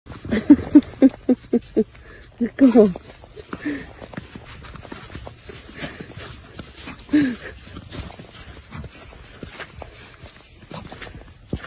Gundog plays around in the snow as Scotland wakes up to arctic chill
Following forecasters' prior indication that there may be a probability of snowfall in London, fresh snow and ice warnings have been issued for the United Kingdom today.

As temperatures drop below zero, an Arctic chill is expected to bring further "hazardous" snow and ice to large areas of Northern and Southern England as well as Scotland by at least Friday.

The Met Office has issued and extended yellow warnings today, affecting the entire eastern coastline as far south as Cornwall and extending from the top of the nation. Tomorrow, they will be in the South from 3 a.m. until 4 p.m.